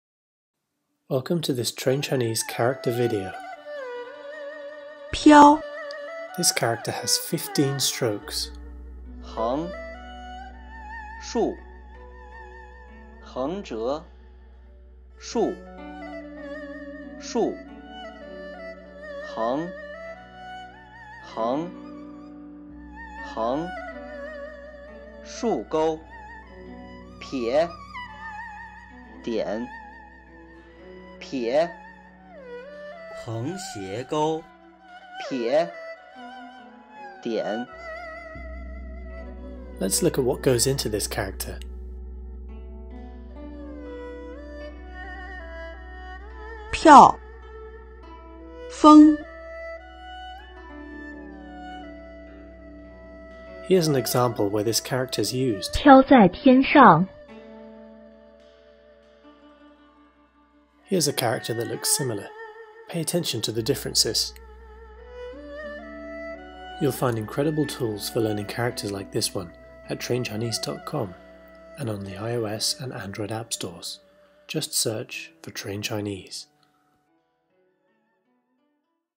How to write 飘 (piāo) – to float in the air – stroke order, radical, examples and spoken audio
Learn how to write 飘. 飘 (piāo) is a Chinese character meaning "to float in the air". or in our apps – just search "trainchinese" in the iOS or Android app stores. This character is not encountered frequently in Mandarin Chinese.It's from Revised HSK Level 5, the second-highest HSK level.